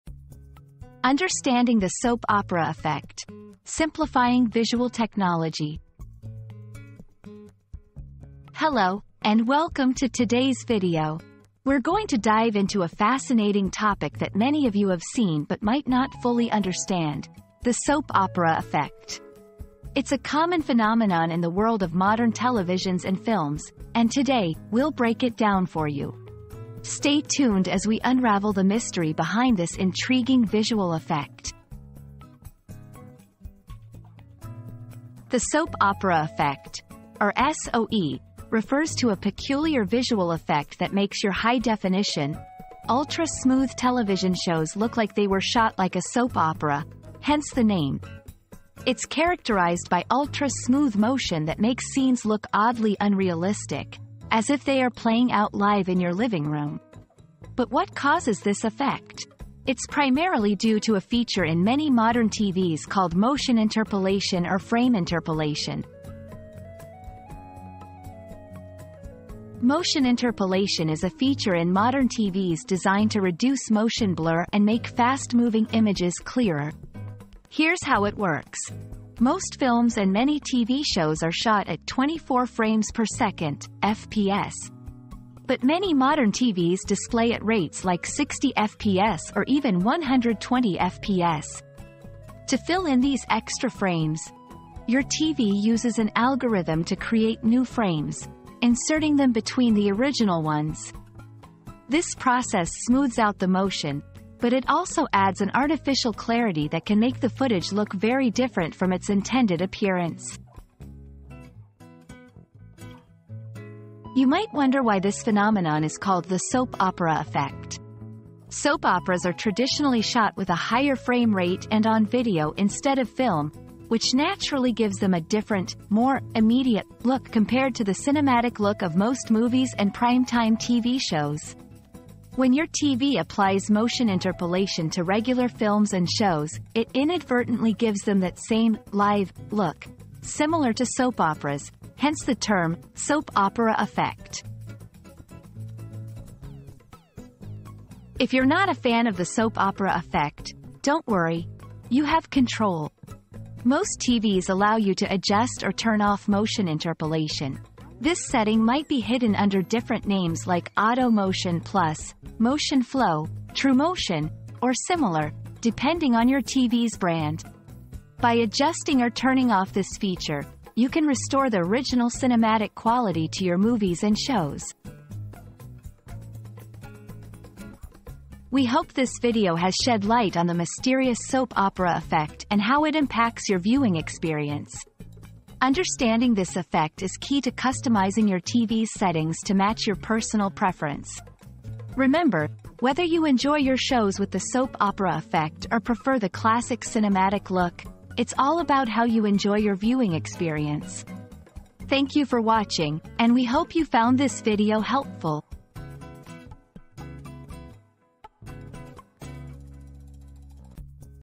Understanding the Soap Opera Effect: Simplifying Visual Technology
Unraveling the Soap Opera Effect: Decoding Visual Technology • Discover the secrets behind the Soap Opera Effect and gain a deeper understanding of visual technology. Join us as we simplify this phenomenon and shed light on its impact on your viewing experience.

00:00 • Introduction - Understanding the Soap Opera Effect: Simplifying Visual Technology
00:34 • What is the Soap Opera Effect?
01:09 • The Role of Motion Interpolation
01:53 • Why is it Called the Soap Opera Effect?
02:27 • How to Control the Soap Opera Effect